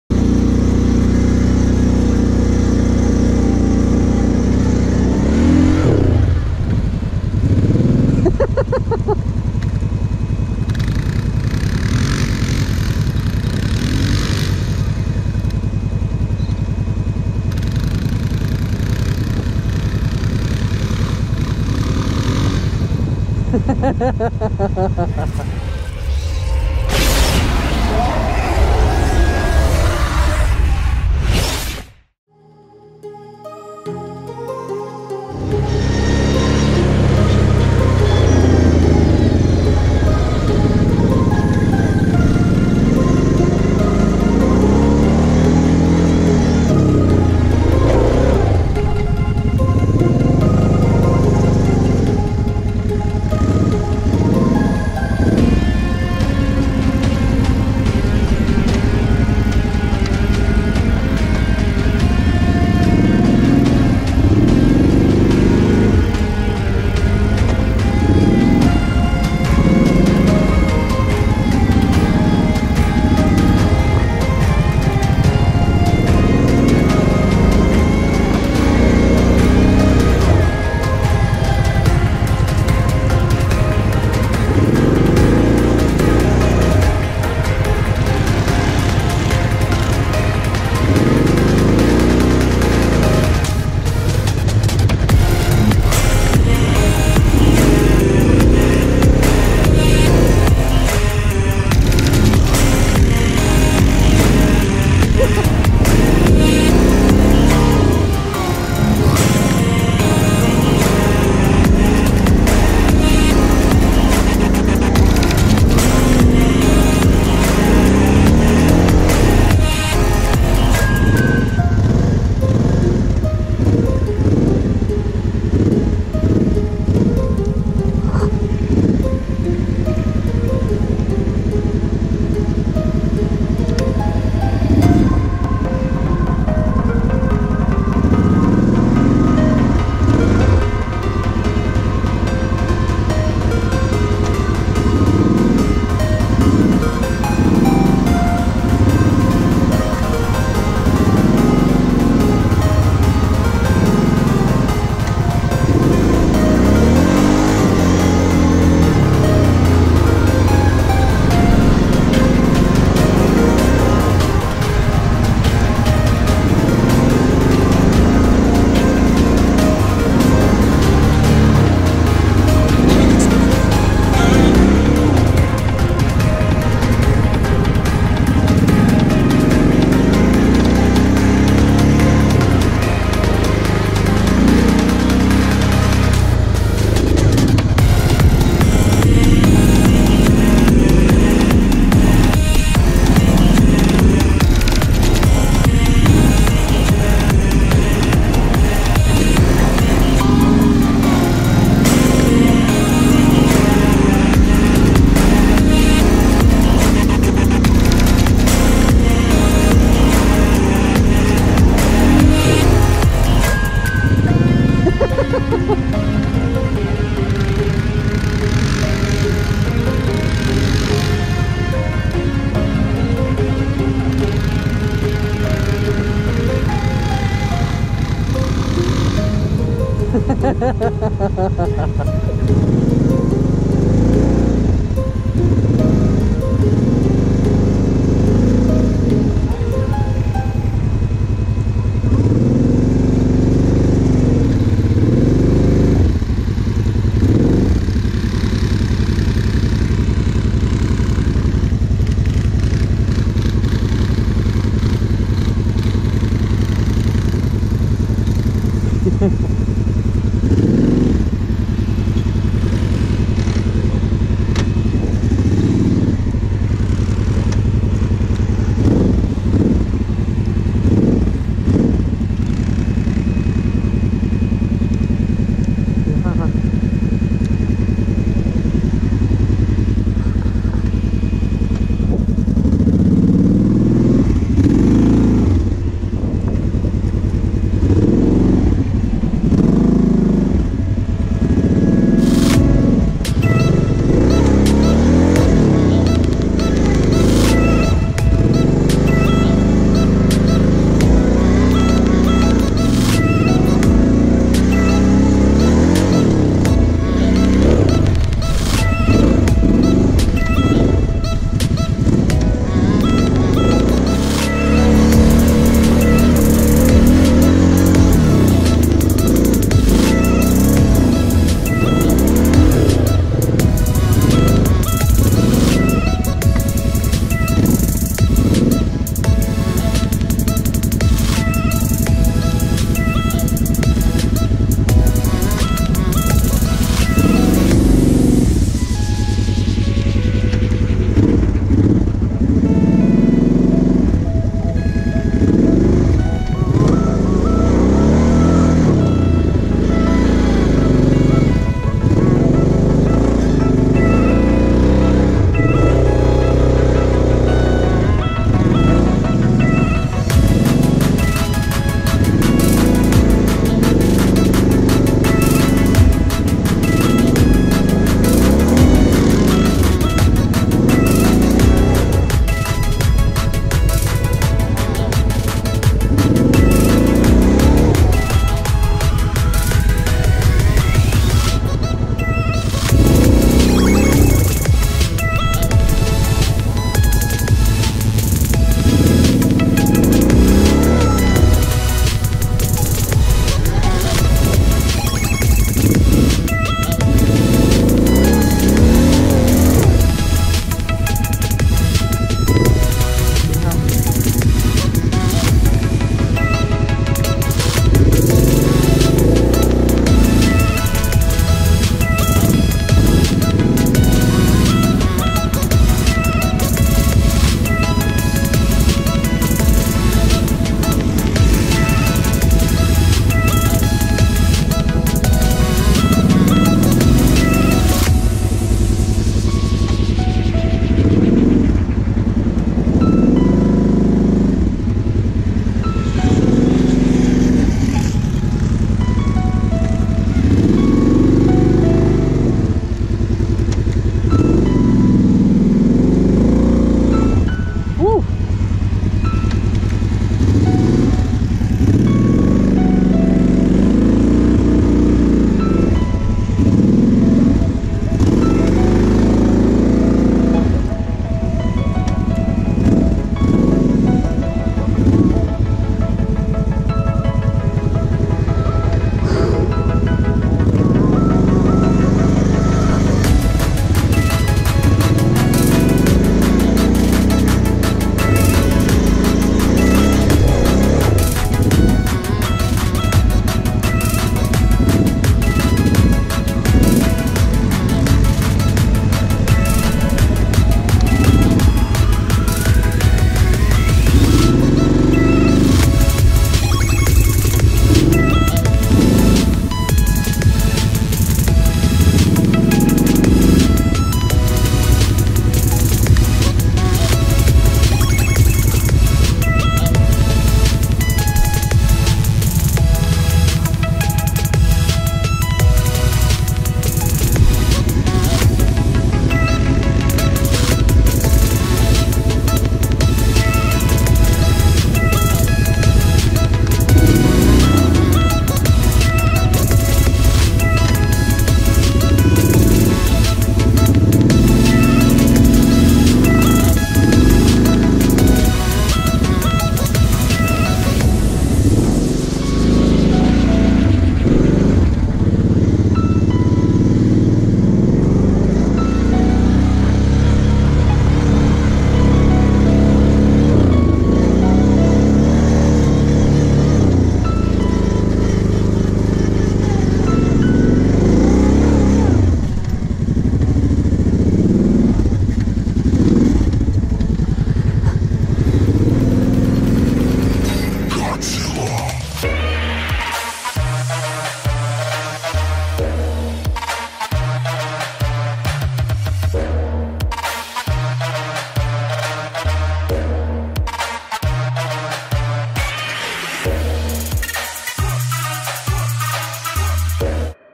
Kawasaki KFX 700 V-Force | Kymco Maxxer 250 | Quad fahren in Münster
Hier mal ein paar ältere Aufnahmen aus dem August 2018.
Brauche Platz auf der Festplatte, und es wäre schade wen ich es einfach nur lösche.

More Quad-YouTuber:
- Kevoconner OCNR

Follow me and my people too:
Instagram Biomorph: